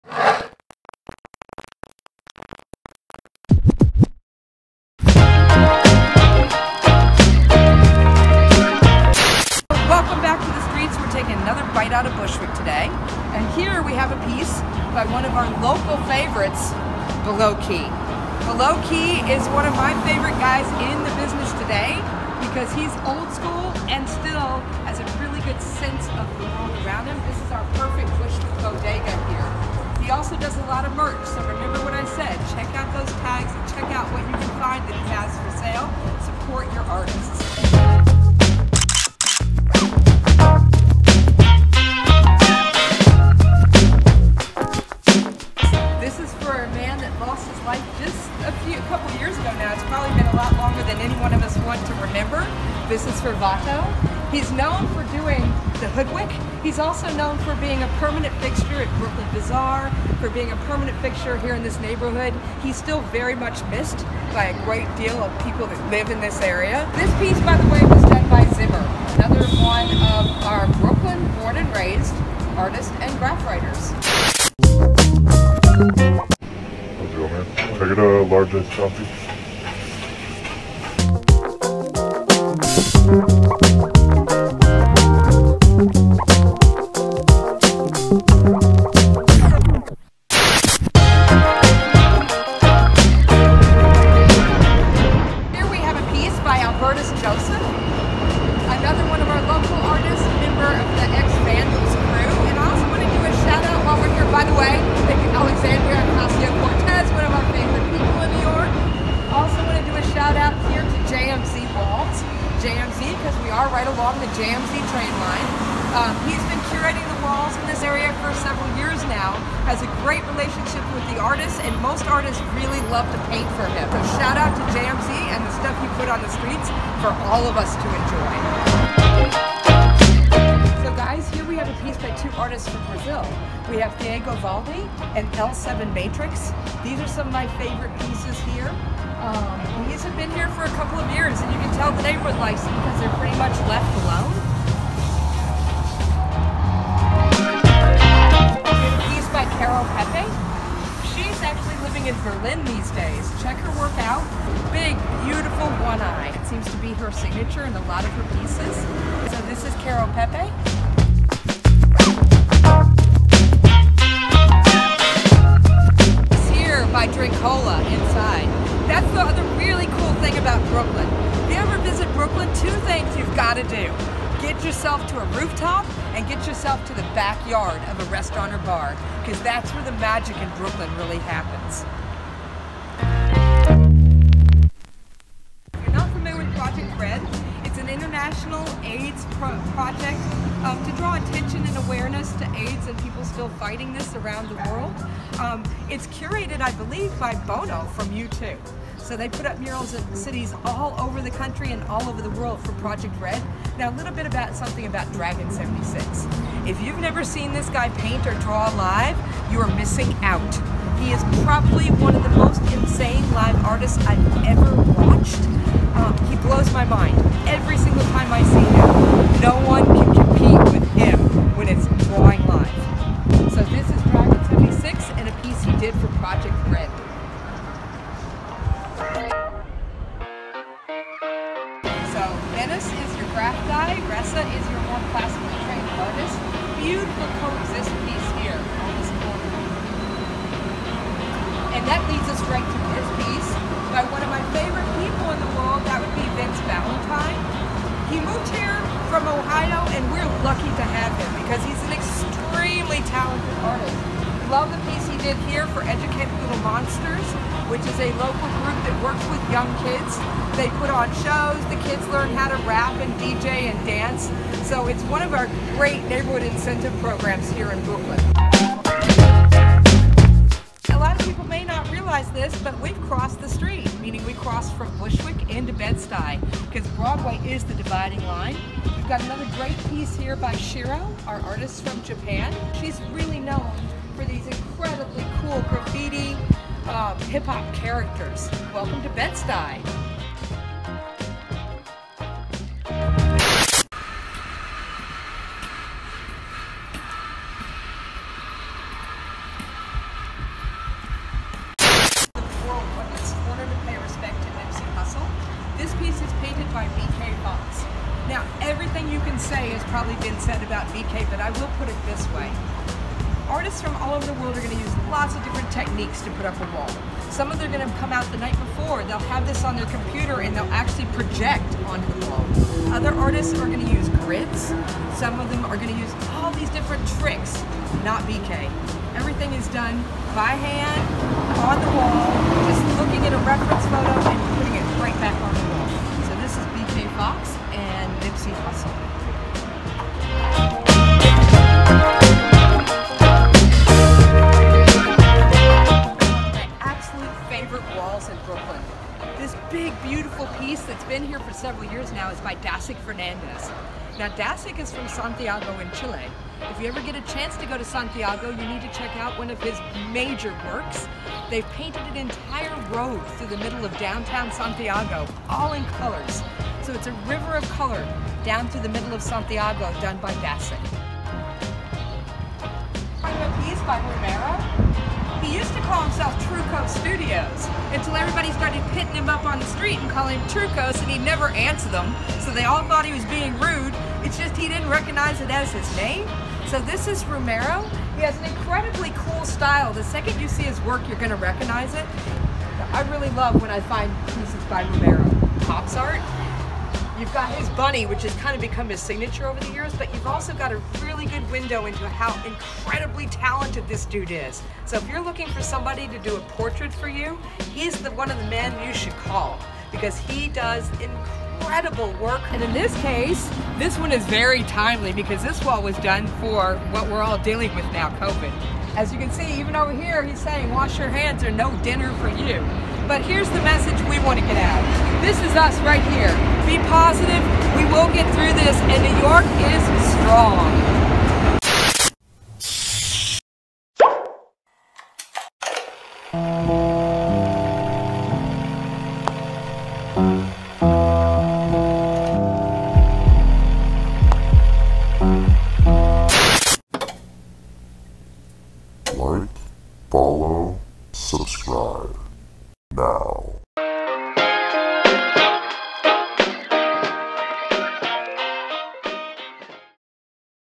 StreetBytesNYC | Premiere | Welcome to StreetBytes
Welcome to StreetBytes NYC. Joins us as we take a tour of some amazing street art.

Artists featured in this episode:

Belowkey @belowkey
Zimer @zimernyc
Albertus Joseph @albertusjoseph
JMZ @jmzwalls
Thiago Valdi @thiagovaldi
L7 Matrix @l7matrix
Caro Pepe @caro.pepe
Eric Drinkala @drinkala
Dragon76 for Project Red @dragon76art
Menace & Resa @menaceresa
Vince Ballentine for Educated Little Monsters @VBallentine99
Shiro @shiro_one
BK Foxx @bkfoxx
Dasic Fernandez @dasicfernandez
Ramiro @ramirostudios
Hops @hopsart_1